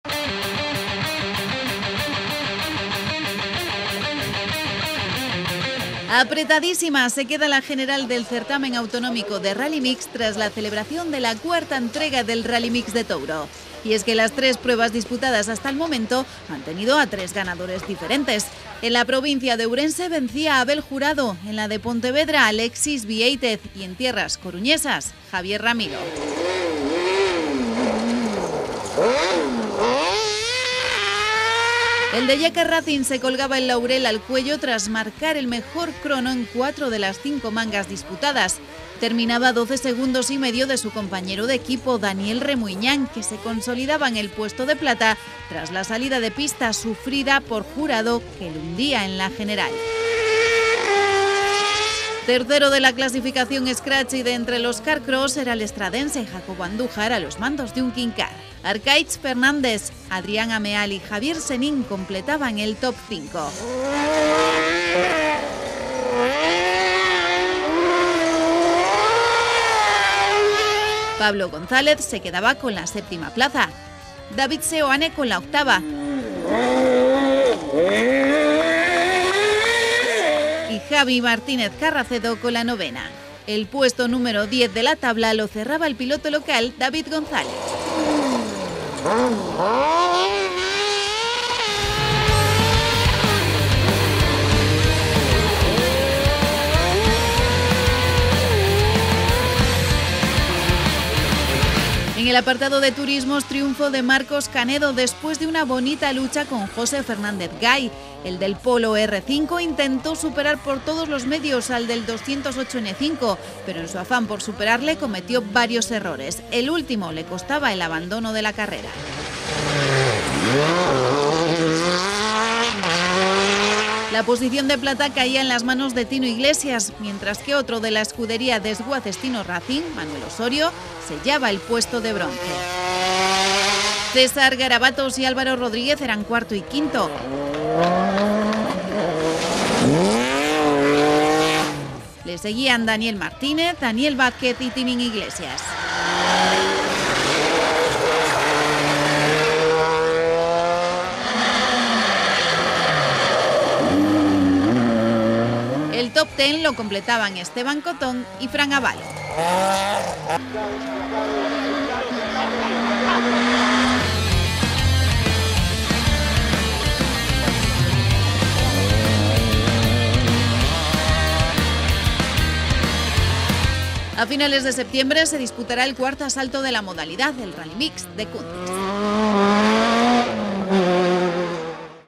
4º RALLYMIX TOURO 2021 | By CdRas
Apretadísima se queda la general del certamen autonómico de Rallymix tras la celebración de la cuarta entrega del Rallymix de Touro. Y es que las tres pruebas disputadas hasta el momento han tenido a tres ganadores diferentes. En la provincia de ourense vencía Abel Jurado, en la de Pontevedra Alexis Vieitez y en tierras coruñesas Javier Ramilo.

El de Yacar Racing se colgaba el laurel al cuello tras marcar el mejor crono en 4 de las cinco mangas disputadas. Terminaba a 12 segundos y medio de su compañero de equipo, Daniel Remuiñán, que se consolidaba en el puesto de plata tras la salida de pista sufría Jurado y lo hundía en la general.

Tercero de la clasificación scratch y de entre los kart-cross era el estradense Jacobo Andújar a los mandos de un kinkar.

El resto del top ten era copado al completo por vehículos no carrozados. Arkaitz Fernandes, Adrián Ameal y Javier Senín completaban el top 5. Pablo González se quedaba con la séptima plaza, David Seoane con la octava y Javi Martínez Carracedo con la novena. El puesto número diez de la tabla lo cerraba el piloto local David González.

En el apartado de turismos triunfo de Marcos Canedo después de una bonita lucha con Jose Fernández Gay. El del Polo R5 intentó superar por todos los medios al del 208 N5 pero en su afán por superarle cometió varios errores, el último, le costaba el abandono de la carrera.

La posición de plata caía en las manos de Tino Iglesias mientras que otro de la Escudería Desguaces Tino Racing, Manuel Osorio, sellaba el puesto de plata.

César Garabatos y Álvaro Rodríguez eran cuarto y quinto. Le seguían Daniel Martínez, Daniel Vázquez y Tinín Iglesias.

El top ten lo completaban Esteban Cotón y Fran Abalo.

A finales de septiembre se disputará el cuarto asalto de la modalidad, el Rallymix de Cuntis.